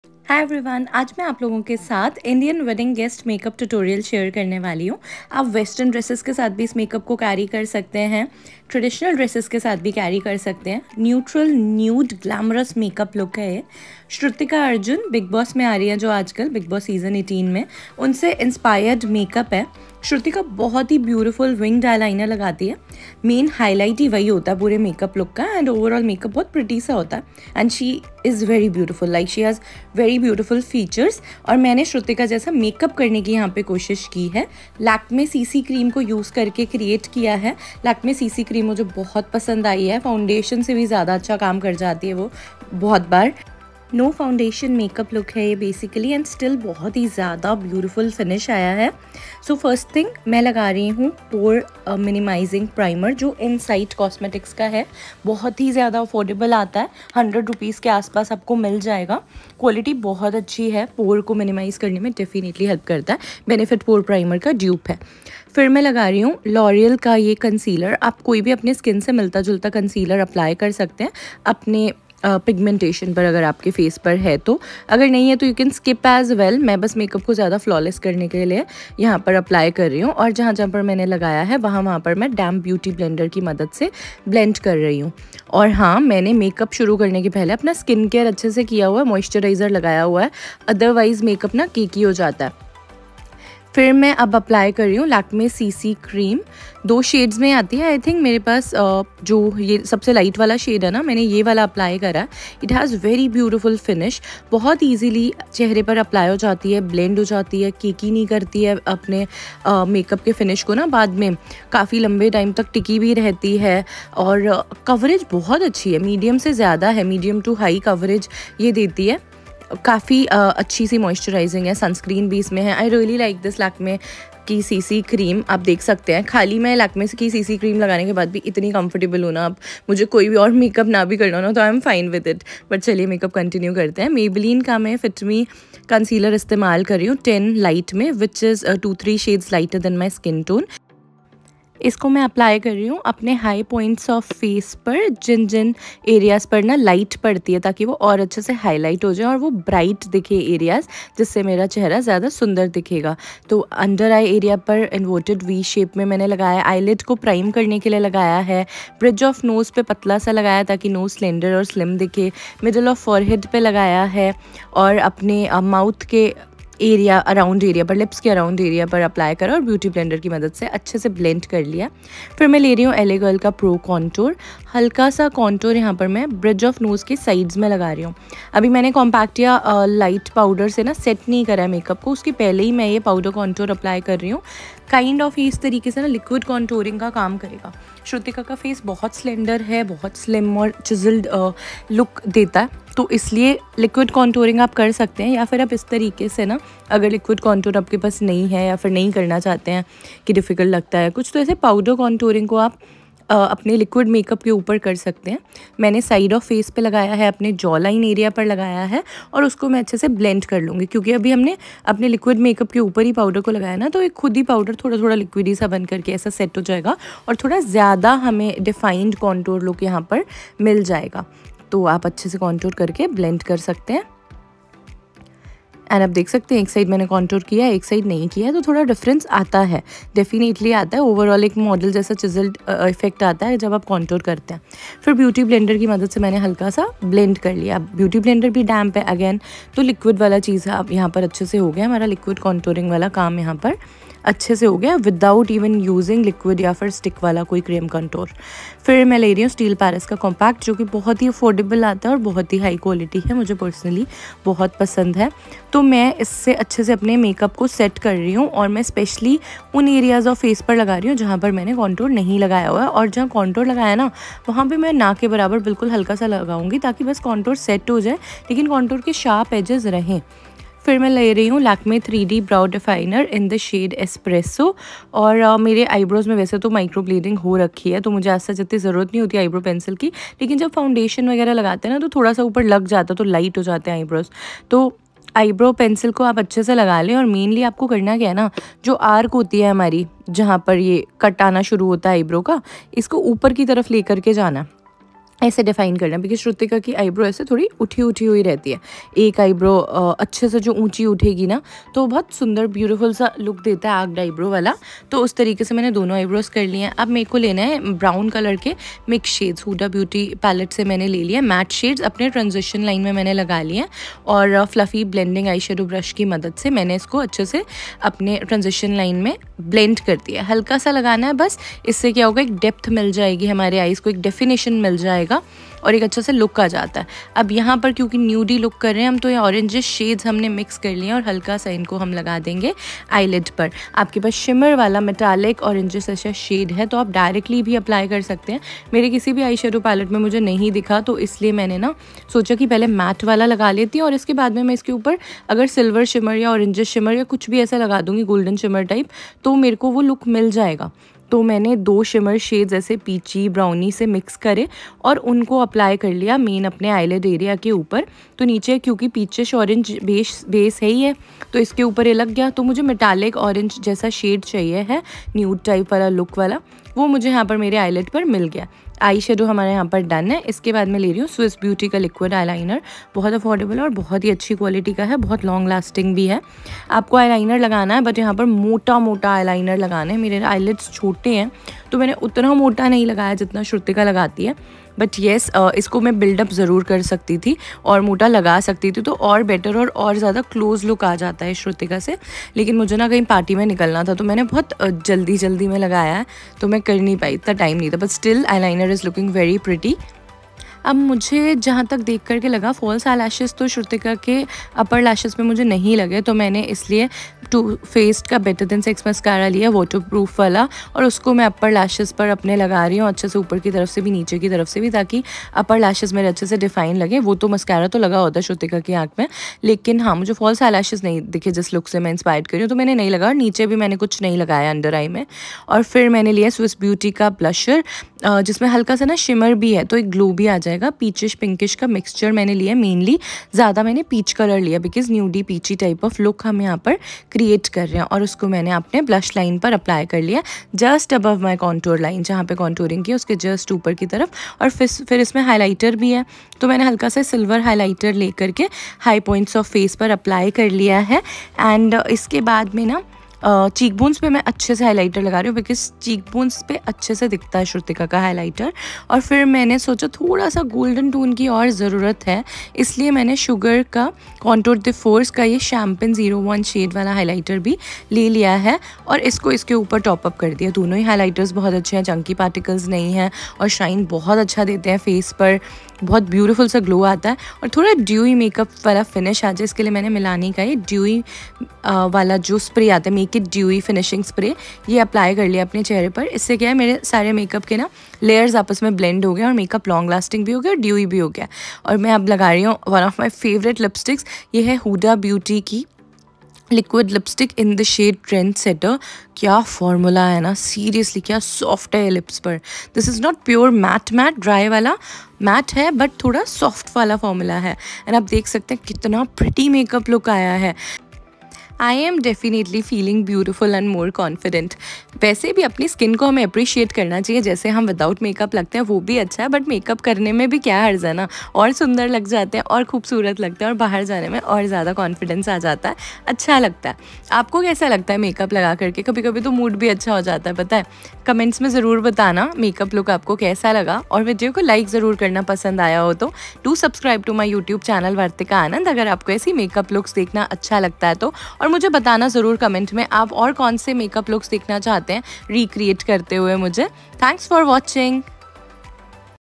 Lakme CC Cream Full Face Makeup Tutorial No Foundation | Indian Wedding Guest Shrutika inspired
Lakme CC Cream | No foundation Makeup |
Shrutika Arjun Big Boss 18 inspired Makeup | Indian Wedding Guest Makeup Tutorial in Hindi for Beginners

Insight Pore Minimizer Primer
L’oreal Infalliable Full Wear Full Coverage Concealer 314 Beige
Lakme 9 to 5 CC Cream SPF 30 ++ 01 Beige
Steel Paris Compact Powder 03
L.A. Girl PRO Contour Powder Highlight Contour Tan
Lakme 3D Brow Definer Espresso
Huda Beauty Eyeshadow Palette
Focallure Endless Possibilities Eyeshadow Palette
Swiss Beauty Liquid Eyeliner
Too Faced Waterproof Mascara
Swiss Beauty Blushing of Love Baked Blusher 02
Sugar Contour De Force Mini Highlighter 01 Champagne Champion
Milani Make it Dewy Setting Spray
Huda Beauty Liquid Lipstick Trendsetter